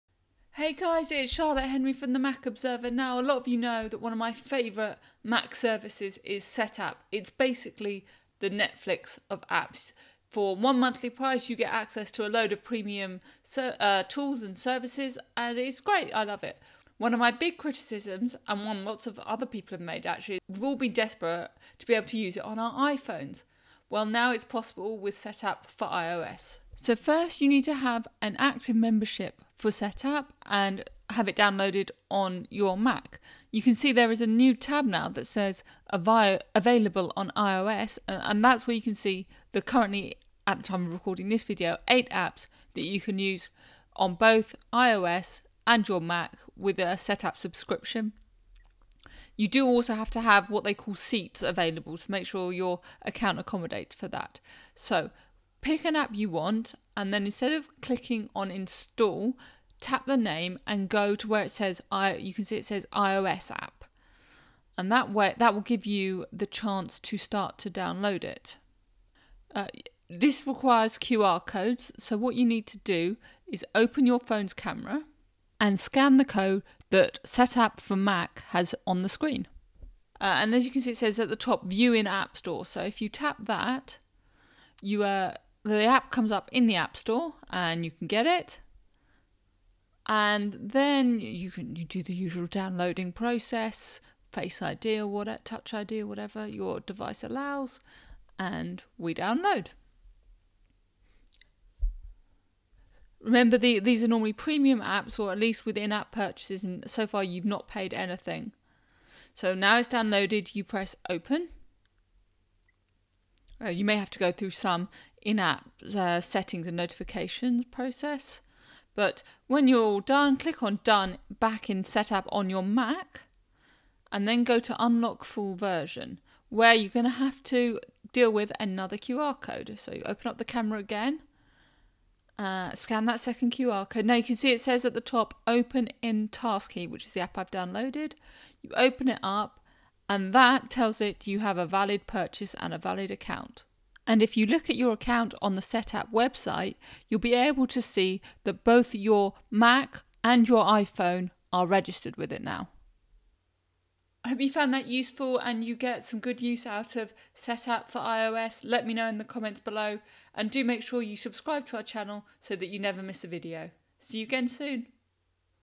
Use Setapp on Your iPhone
You need an active Setapp subscription that gives you access to more than one device, called 'seats'. With that done, open the Setapp app on your Mac. There is now a new 'Available in iOS' tab at the bottom. It shows which apps you can access on your iPhone via your Setapp subscription.

Click on the app's name/description. You then get an option for the iOS version.

The first stage is using your iPhone camera to read a QR code provided by Setapp.

With the download complete, you may need to go through some app-specific settings like notifications. When that's done, return to Setapp on your Mac. Tap 'Done' under the first QR code and click 'Unlock Full Version'.

You have a second QR code to scan. If you still have your new app open, swipe up to go back to the home screen and reopen the camera. Scan the second QR code.

Doing this will give you the subscription your entitled to with Setapp.